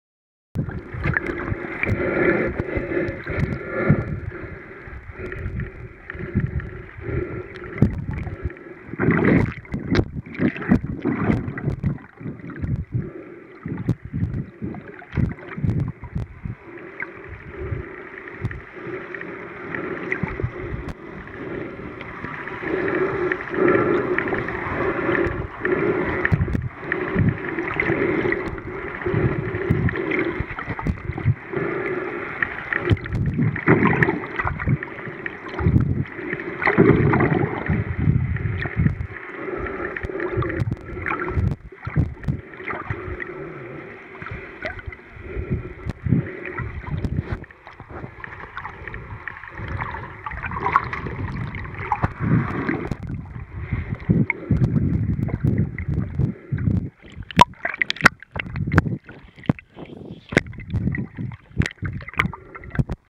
Swimming with Whale Sharks in the Sea of Cortez
Our adventure sighting over 15 whale sharks in one morning and swimming with many of them.